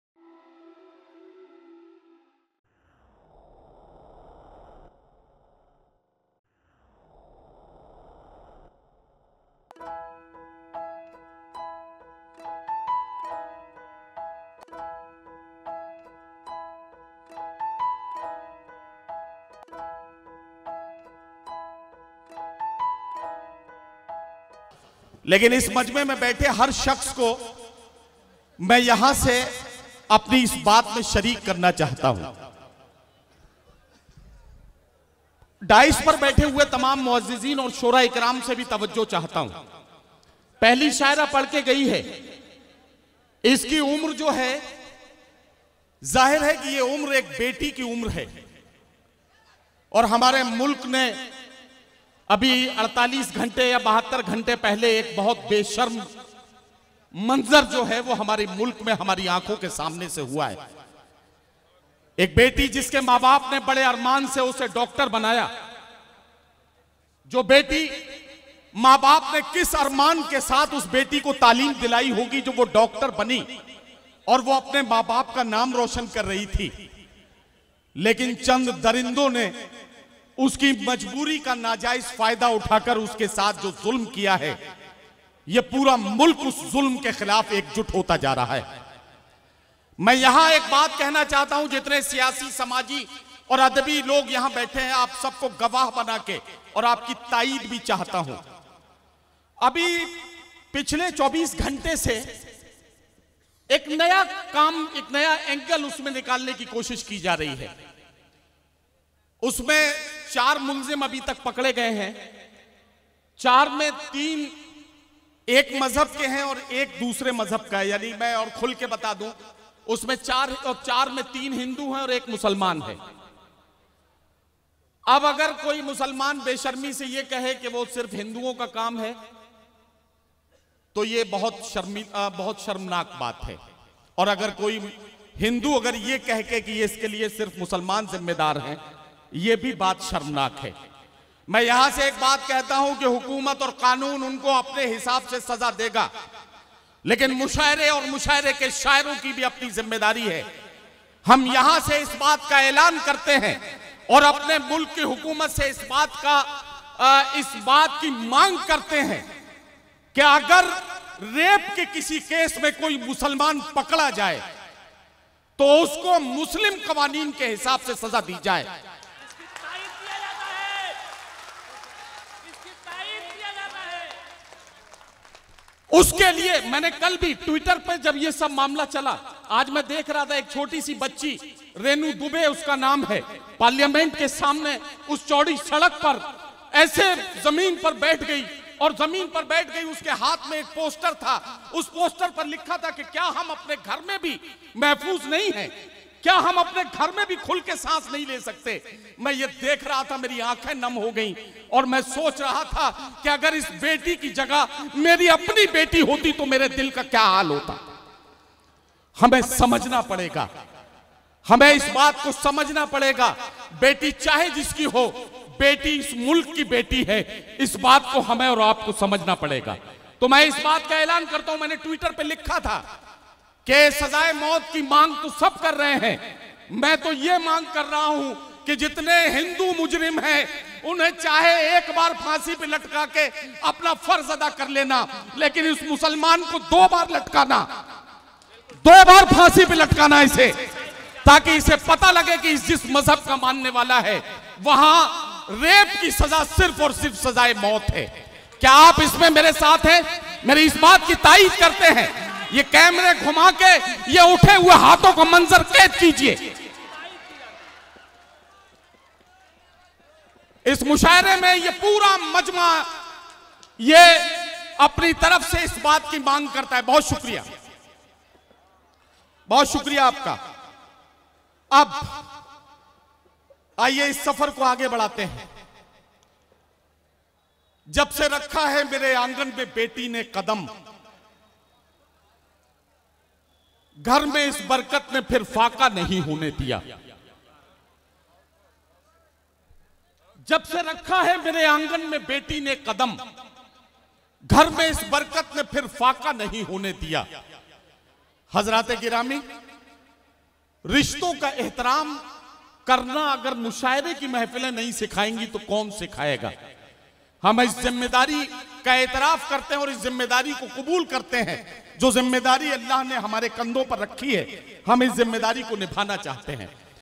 Nadeem Farrukh All India Mushaira Kavi Sammelan Motihari 2019 Con Mohibbul Haque JK Mushaira Media
Nadeem Farrukh
All India Mushaira Kavi Sammelan Town Hall Motihari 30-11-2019
Nizamat: Nadeem Farrukh
Sadarat: Mohibul Haque
Convenor: Mohibul Haque
Mushaira Committee: Wasil Ahmad Khan
Sajid Raza  Dr. Kumkum Sinha
Dr Saba Akhtar  Gulrez Shahzad
Dr. Abdul Khabir Dr Minatullah
Dr Umesh Akhtar Khurshid Alam
Parvez Khan  Ehtesham Ahmad
Anwar Kamal
Venue: Tawn Hall Motihari Bihar